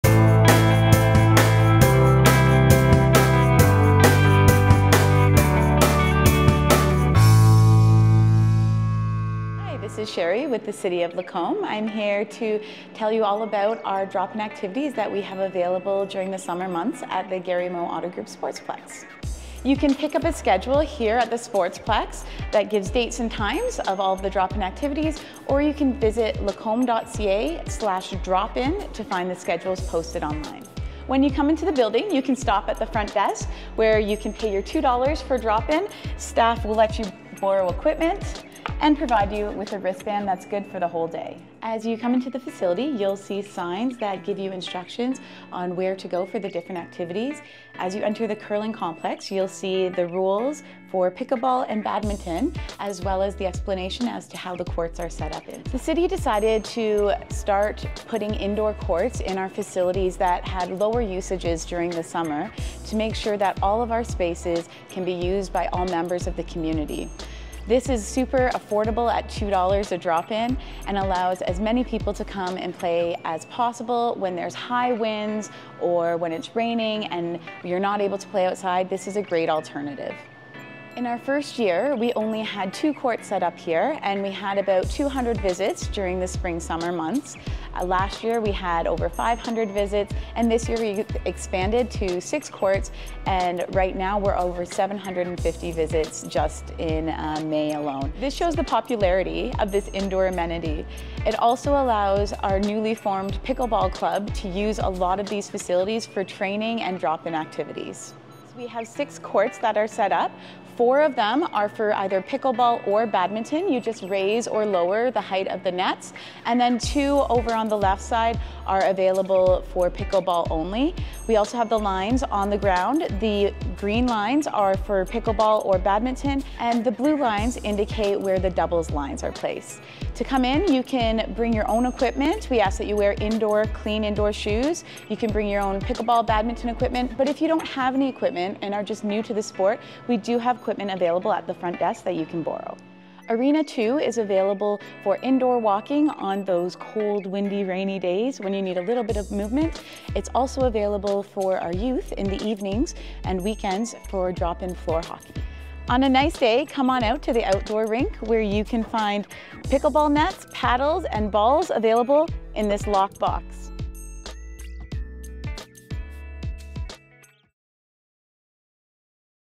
Recreation| City offers floor hockey, pickleball, indoor walking and badminton
The City of Lacombe is excited to invite residents to enjoy a variety drop-in fitness activities at the Gary Moe Auto Group Sportsplex.

Echo Internet Arena and the Curling Complex are now busy summer go-to recreational facilities with full schedules of drop-in activities.

The Curling Rink has six pickleball/badminton nets, calling fans of these racket sports to play from May through August. In the Echo Internet Arena, residents can go for a stroll for scheduled indoor walking or participate in the ever-popular game of floor hockey.

Schedules for all drop-in activities are on the City's dedicated drop-in webpage, Lacombe.ca/dropin.

To accommodate the fast-growing sport of pickleball, the Outdoor Rink behind the Sportsplex is now home to four courts for public use. Nets, paddles, and balls are available at the Sportsplex's front desk. More information is also available from the Lacombe Pickleball Club

Residents are encouraged to take advantage of these offerings and to keep track of City recreational programming at Lacombe.ca and through the City's social media channels.

Come out, get moving, and have fun at the Gary Moe Auto Group Sportsplex this summer.